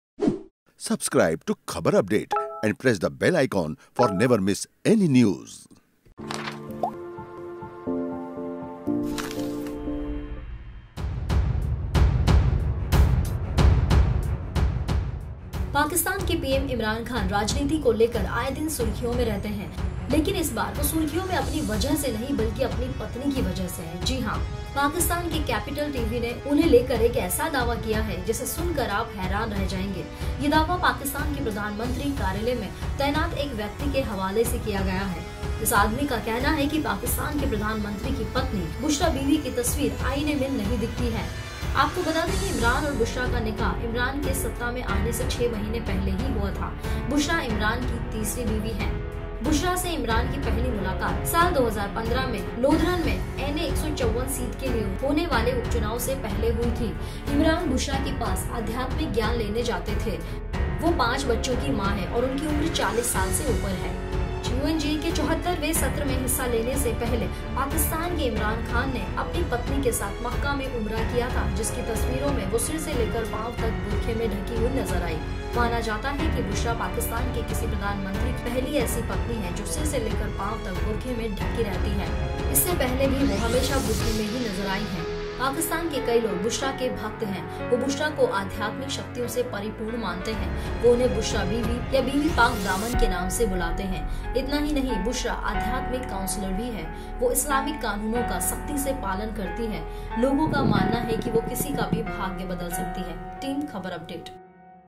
Pak Channel का दावा- शीशे में नहीं दिखती Pak PM की बीवी की तस्वीर | Khabar Update
पाकिस्तान के पीएम इमरान खान राजनीति को लेकर आये दिन सुर्खियों में रहते है. लेकिन इस बार वो सुर्खियों में अपनी वजह से नहीं बल्कि अपनी पत्नी की वजह से है. जी हां पाकिस्तान के कैपिटल टी वी ने उन्हे लेकर एक ऐसा दावा जिसे सुनकर आप हैरान रह जाएंगे.यह दावा पाकिस्तान के प्रधानमंत्री कार्यालय में तैनात एक व्यक्ति के हवाले से किया गया है। इस आदमी का कहना है कि पाकिस्तान के प्रधानमंत्री की पत्नी बुशरा बीबी की तस्वीर आईने में नहीं दिखती है. पूरी खबर जानने के लिए देखिये ये वीडियो-